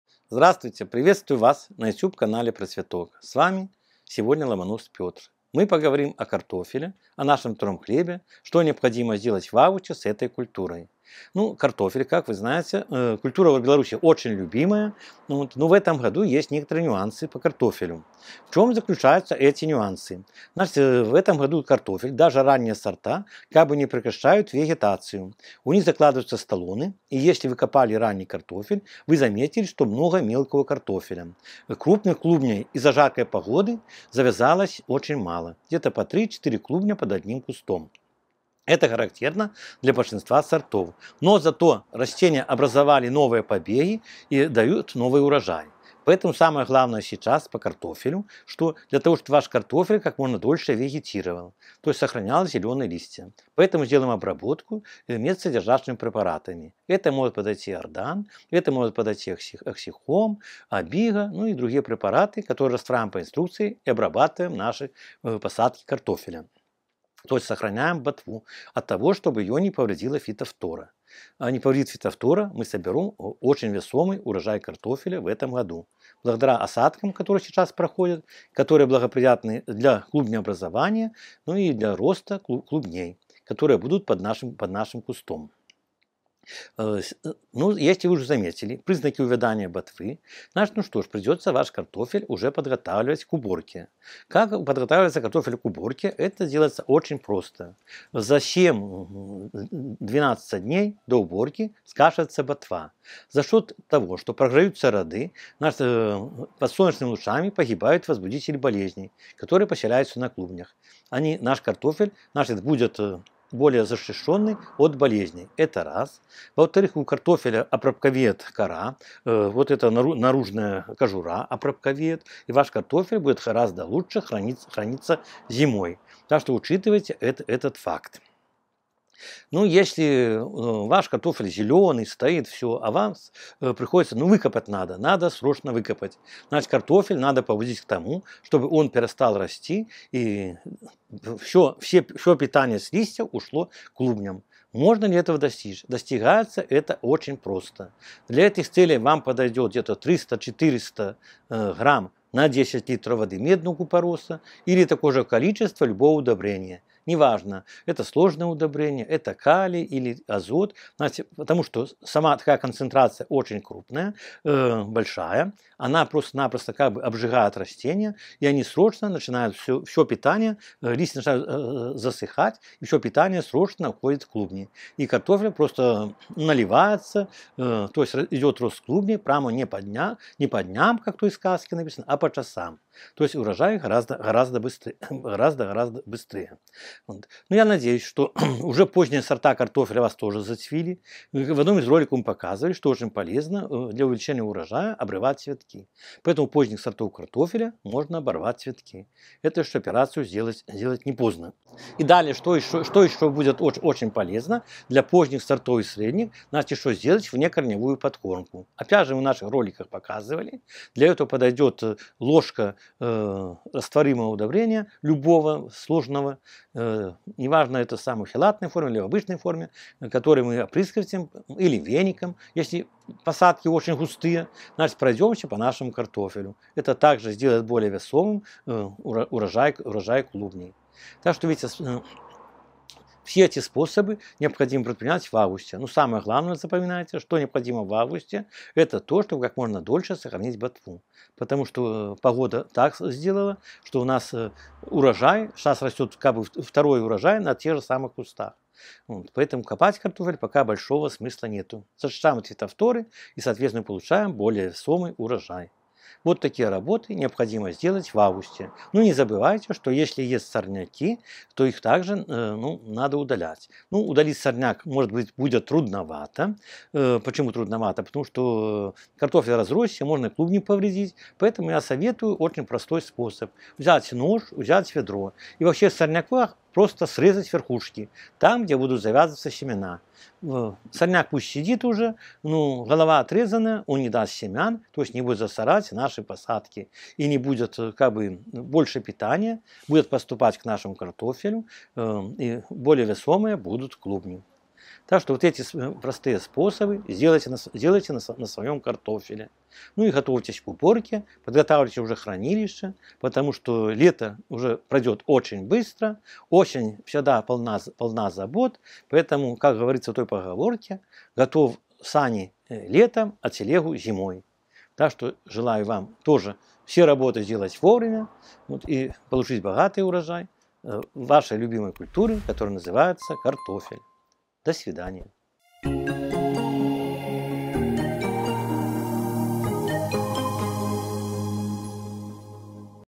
УРОЖАЙ КАРТОФЕЛЯ БУДЕТ ГОРАЗДО БОЛЬШЕ, если сделаете так В АВГУСТЕ!
В Беларуси картофель в 2022 году удивил огородников необычным ростом - в августе он все еще активно вегетирует и наращивает клубни. Для того, чтобы клубней было много и они были крупными, необходимо сейчас правильно обработать и подкормить кусты. О правильном уходе за картофелем в августе в этом видео расскажет ученый-агроном Петр Ломонос.

00:00 Приветствие
00:23 Особенности картофеля в Беларуси в 2022 году
00:50 Чем обработать картофель в августе
01:31 Как подготовить картофель к уборке
02:20 Как заставить картофель созревать быстрее
03:10 Обрываем цветы у позднего сорта картофеля
03:22 Внекорневая подкормка поздних сортов картофеля
04:25 Удаляем сорняки